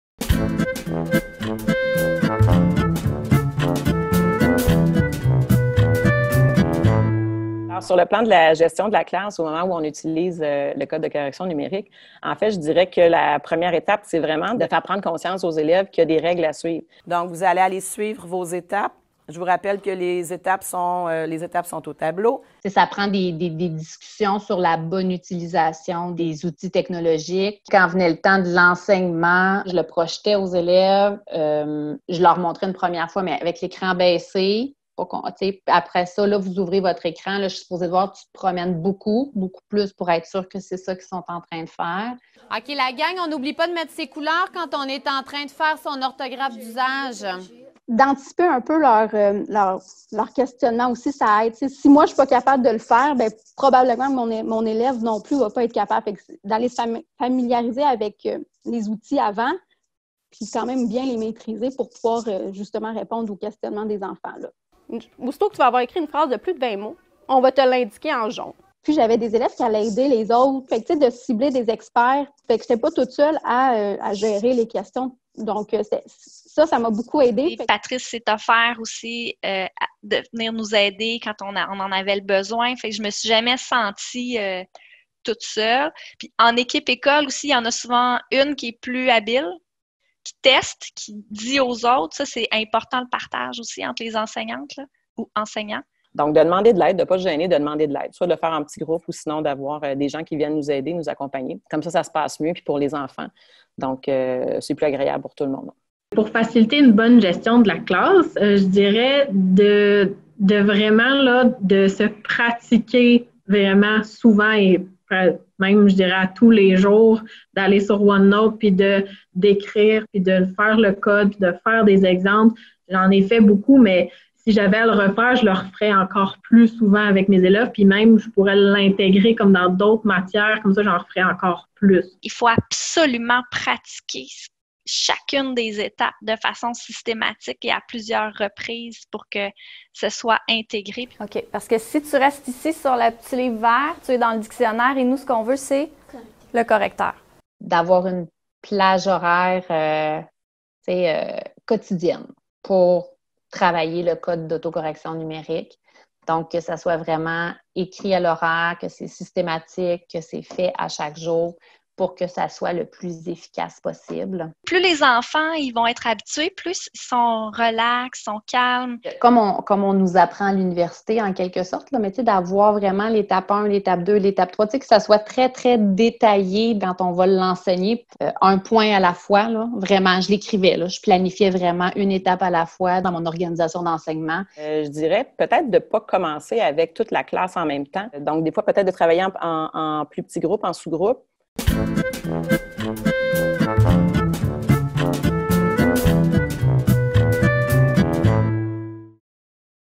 5. Conseils des enseignantes
Dans cette vidéo, les enseignantes donnent leurs  conseils  pour implanter un code d'autocorrection numérique avec les élèves.